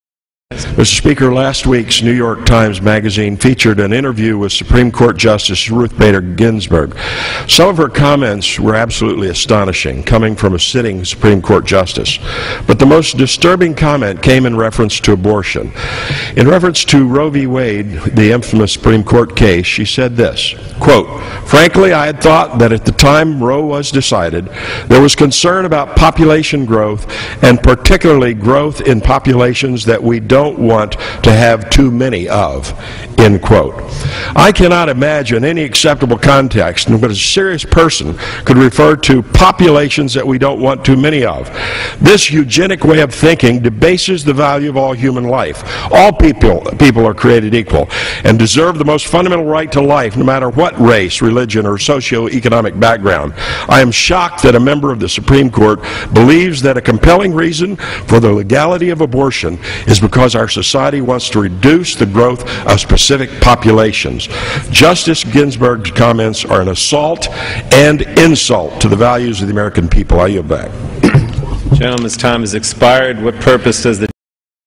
Justice Ginsburg's pro-eugenics views exposed on House floor
Republican Pennsylvania Congressman Joseph R. Pitts (PA-16) exposes pro-eugenics Supreme Court Justice Ruth Bader Ginsburg's comments regarding abortion and human depopulation.